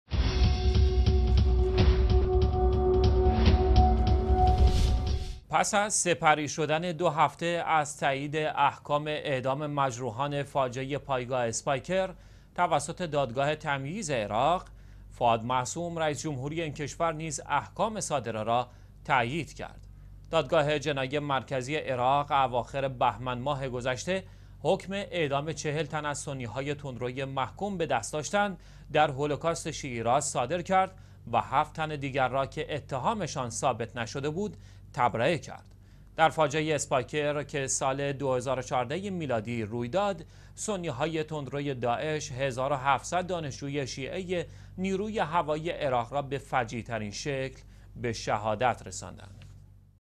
موافقت رئیس جمهور عراق با اعدام محکومان عامل جنایت اسپایکر /۲۵ مرداد ۱۳۹۵
موافقت رئیس جمهور عراق با اعدام محکومان عامل جنایت اسپایکر

پس از گذشت دو هفته از تایید احکام اعدام مجرمان فاجعه اسپایکر توسط دادگاه «تمییز» عراق ، «فواد معصوم» رئیس جمهوریِ این کشور نیز احکام صادره را تایید کرد.

دادگاه جنایی مرکزی عراق اواخر بهمن ماه گذشته، حکم اعدام 40 تن از سنی های تندرویِ محکوم به دست داشتن در هولوكاست شيعى را صادر نموده و 7 تن دیگر را که اتهامشان ثابت نشده بود، تبرئه کرده بود.

در فاجعه اسپایکر که سال 2014 میلادی روی داد، سنی های تند روی داعش، ١٧٠٠ دانشجو ی شيعه نیروی هوایی عراق را به فجیع ترین شکل ممکن به شهادت رساندند.